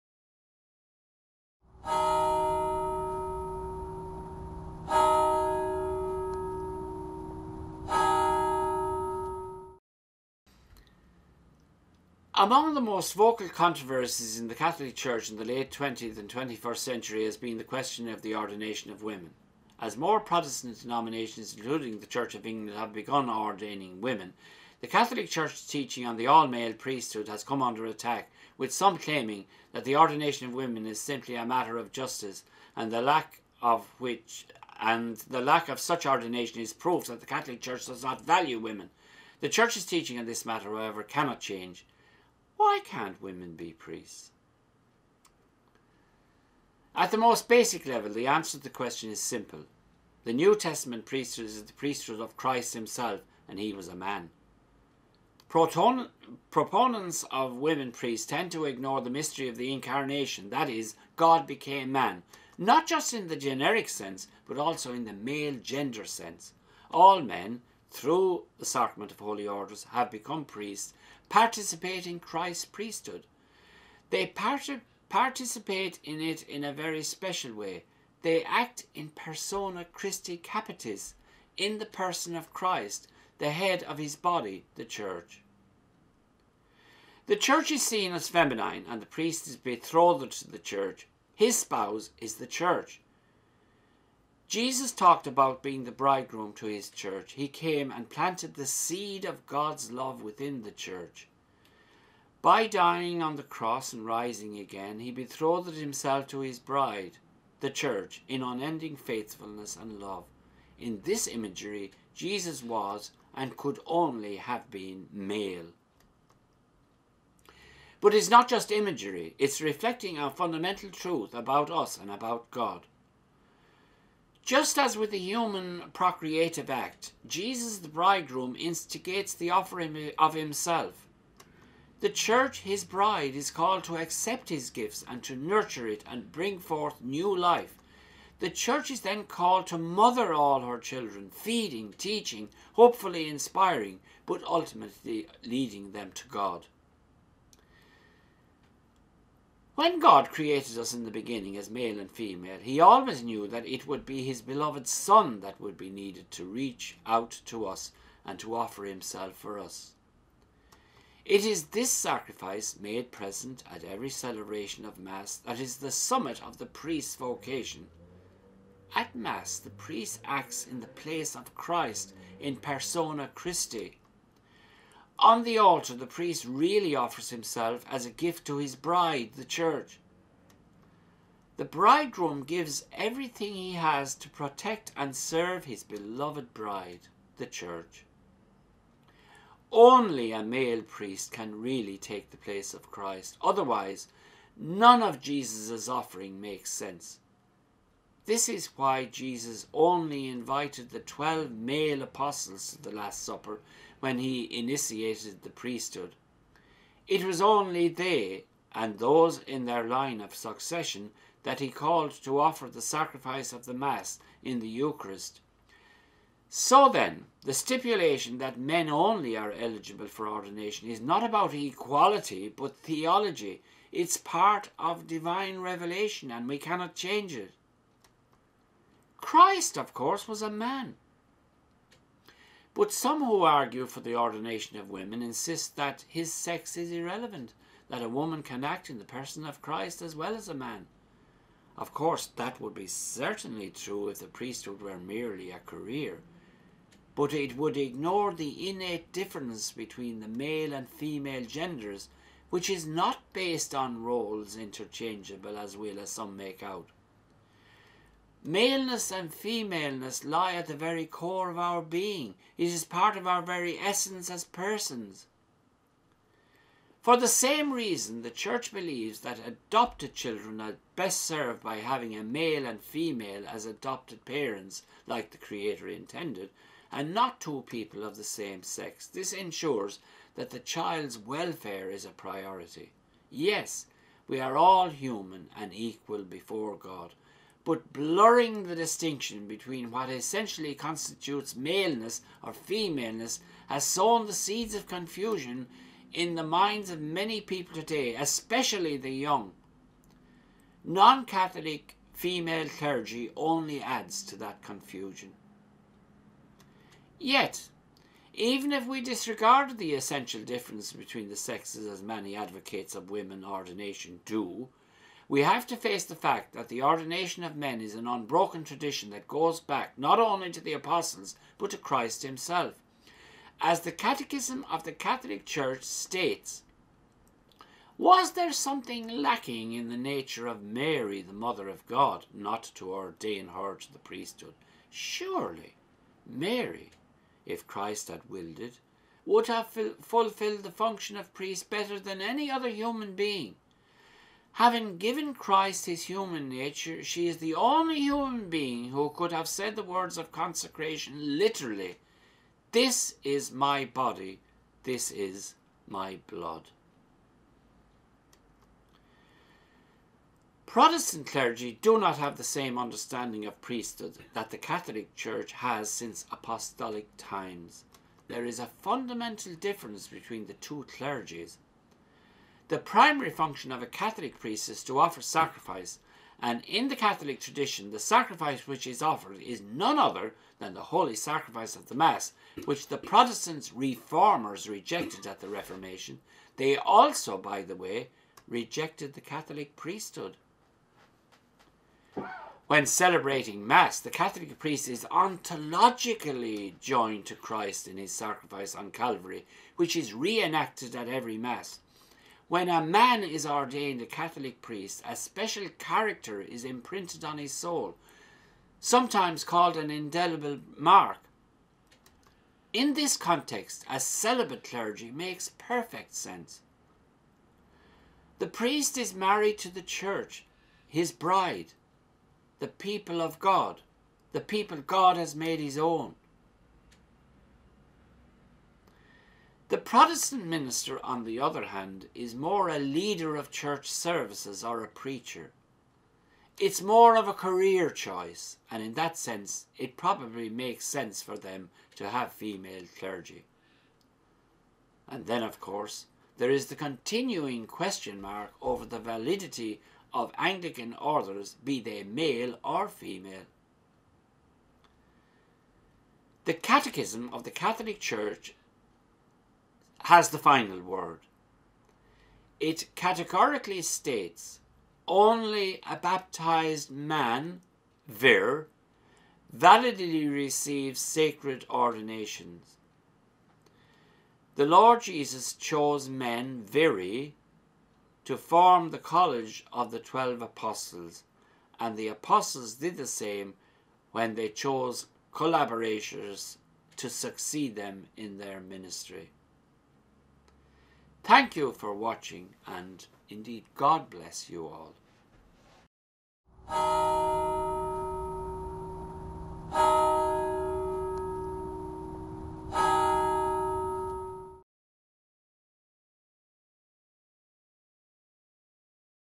The Question of Women Priests
Among the most vocal controversies in the Catholic Church in the late 20th century and early 21st has been the question of the ordination of women. As more Protestant denominations, including the Church of England, have begun 'ordaining women', the Catholic Church's teaching on the all-male priesthood has come under attack, with some claiming that the ordination of women is simply a matter of justice and the lack of such ordination is proof that the Catholic Church does not value women. The Church's teaching on this matter, however, cannot change. Why can't women be priests?